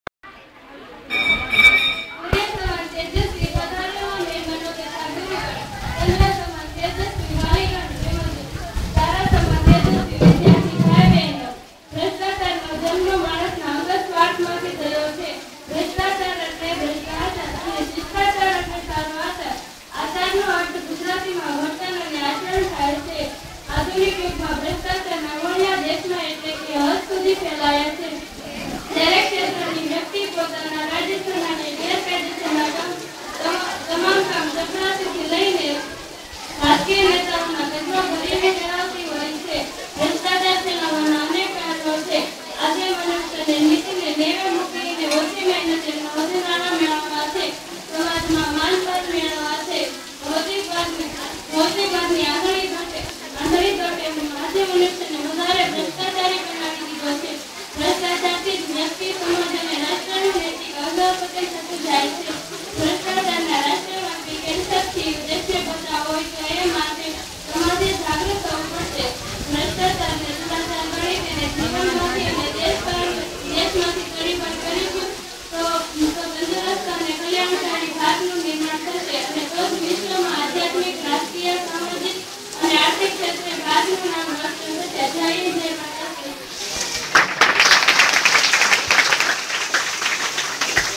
KEEP AWAY CORRUPTION.
15 august celebration in village JIRA.
Student speaks against coruption in school programme.
sardar patel madhyamik school, jira